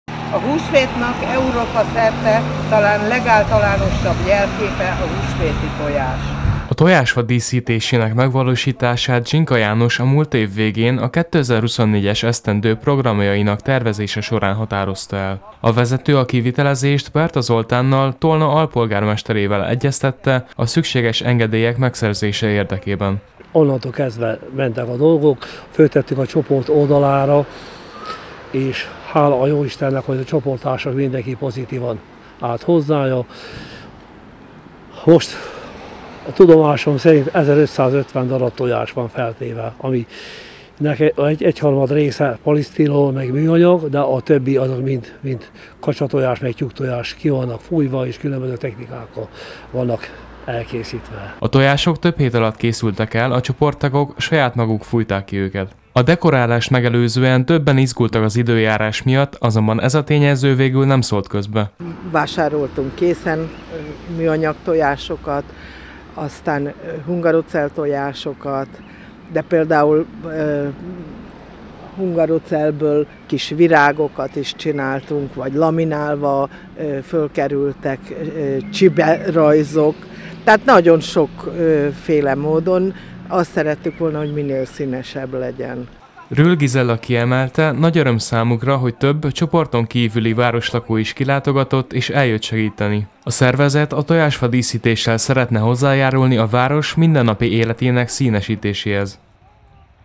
Húsvéti hangulat - tojással díszítették fel a fát Tolnán
Több mint 1500 darab saját készítésű tojással díszített fát Tolnán a Vándorló Szépkorúak Baráti Köre. A csoport vezetője azt mondta, évek óta dédelgetett terve valósult most meg.